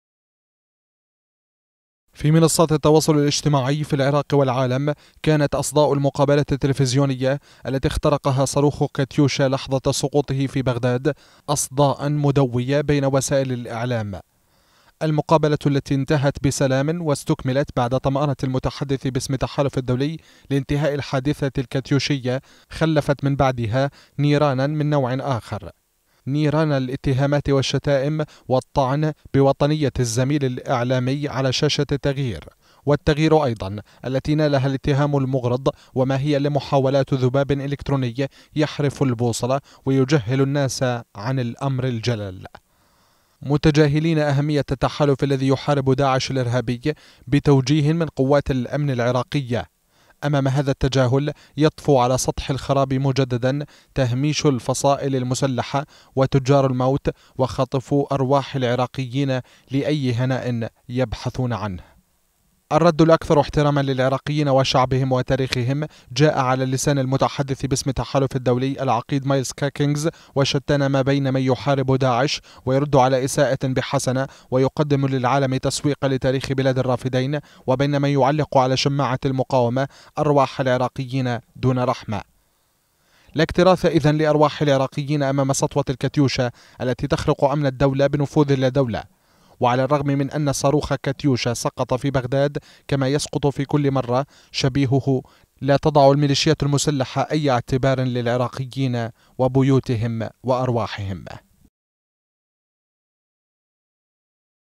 ذباب الميليشيات يستهدف مهنية قناة التغيير | تقرير
المقابلة التي انتهت بسلام واستكملت بعد طمأنة المتحدث باسم التحالف الدولي لانتهاء الحادثة الكاتيوشية، خلفت من بعدها نيران من نوع آخر.. نيران الاتهامات والشتائم والطعن بوطنية الزميل الإعلامي على شاشة التغيير والتغيير أيضا التي نالها الاتهام المغرض وما هي إلا محاولات ذباب الكتروني يحرف البوصلة وتجهيل الناس عن الأمر الأهم.